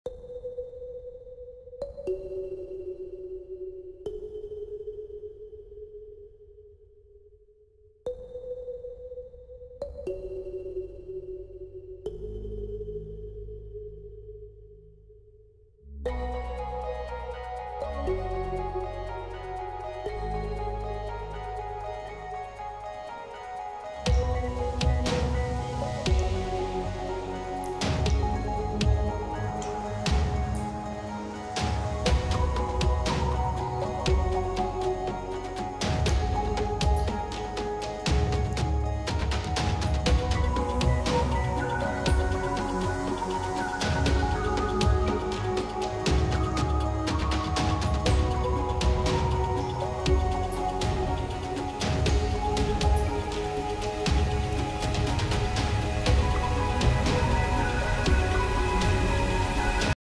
iPad Pro/Procreate Time Lapse: "Flora" Illustration
A one minute time lapse video creating a piece of print artwork titled "Flora" drawn and captured on the Procreate app and the iPad Pro - ©louiedelcarmen.com If you'd like a print of my art go